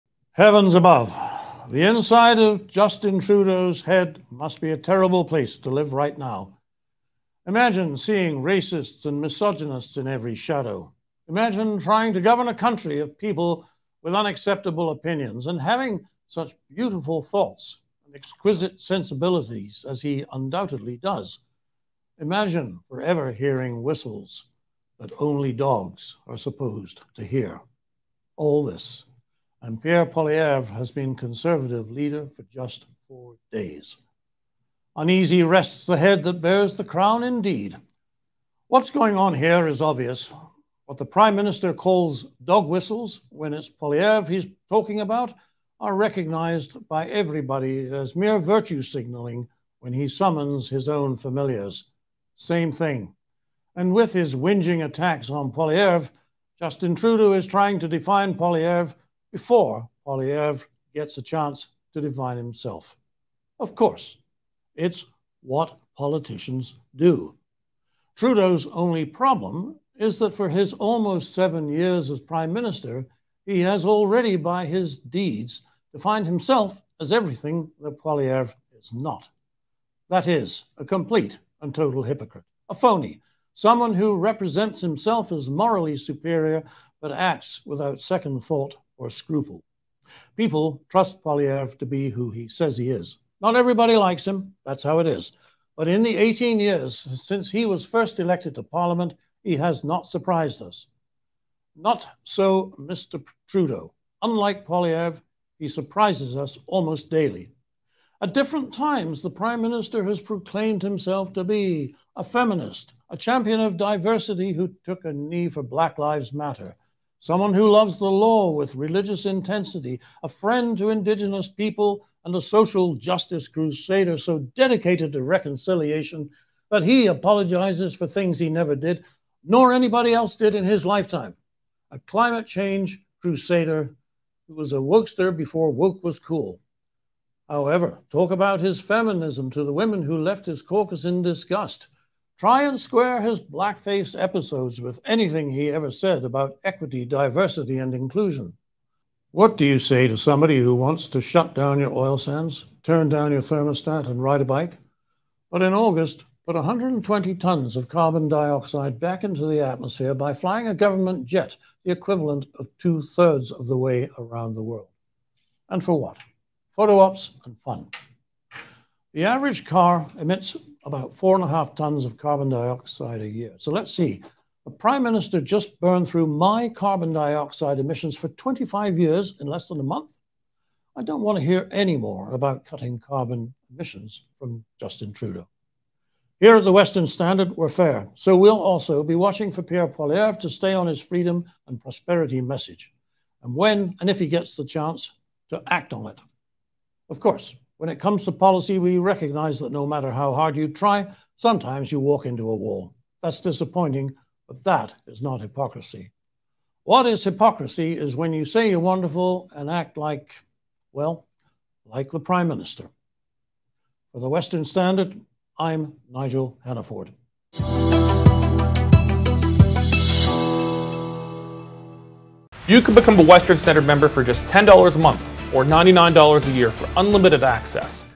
HANNAFORD Poilievre and the many hypocrisies of Justin Trudeau
Justin Trudeau is merely trying to define Poilievre before Poilievre gets a chance to define himself.